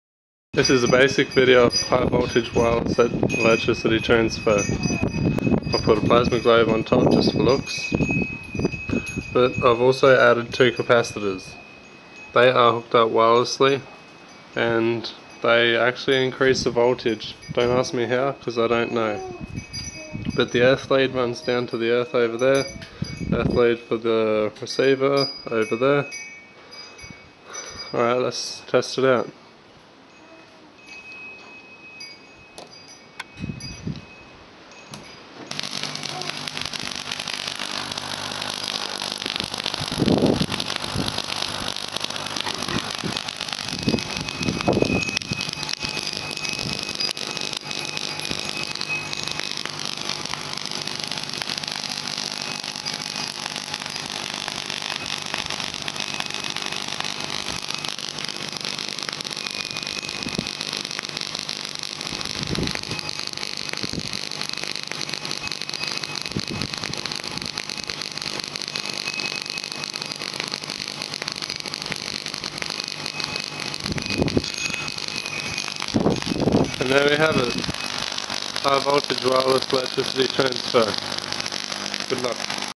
Wireless Electricity, Tesla Coil, Transmitter and Receiver
This is a basic video of a tesla coil and receiver,  I should have put the receiver further away as it overclocks my multimeter.